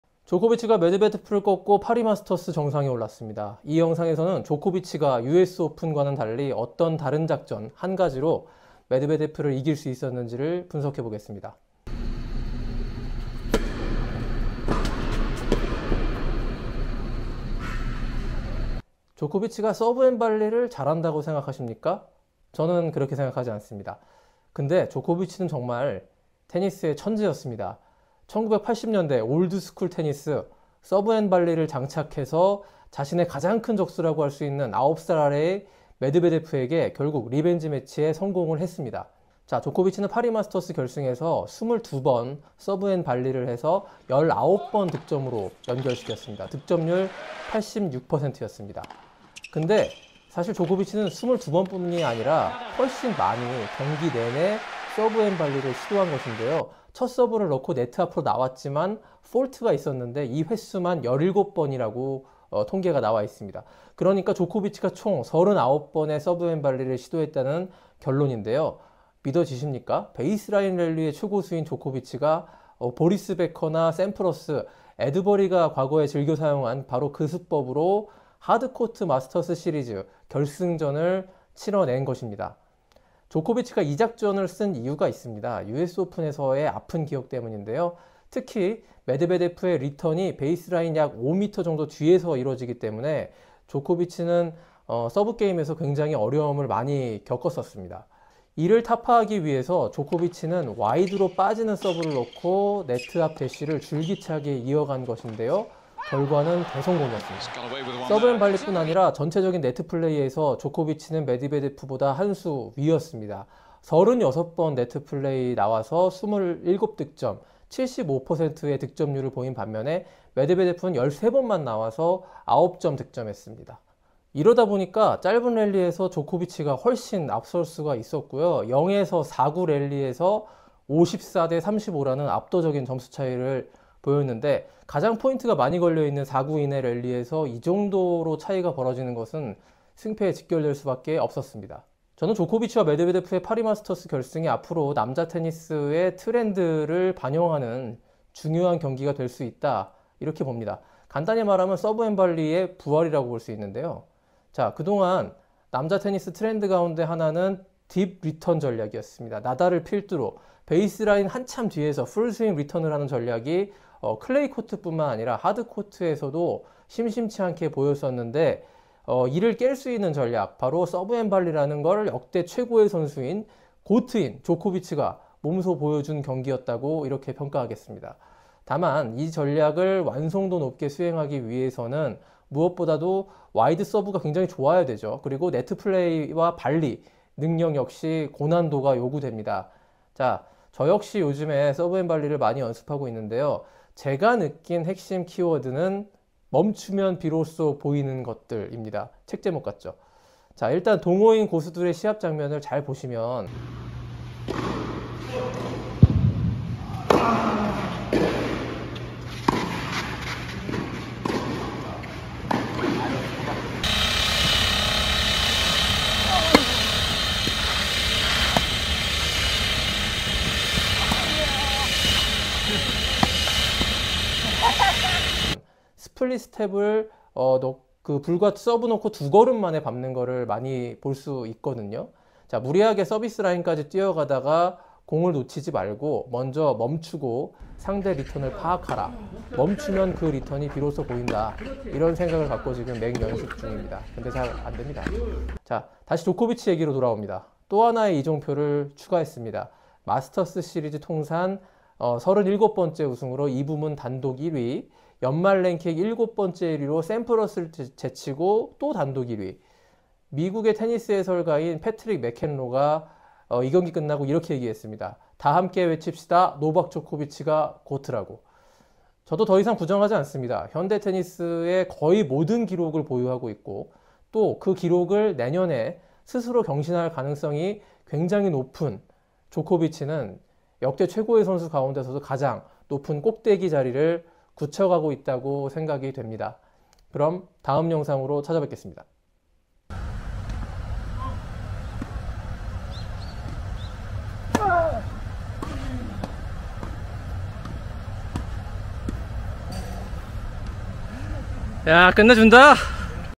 [키키홀릭] 조코비치가 복수혈전에 성공한 이유 "천재적인 서브&발리 전략"
조코 비치가 메드베데프를 꺾고 파리 마스터스 우승. 숨은 비결을 분석합니다!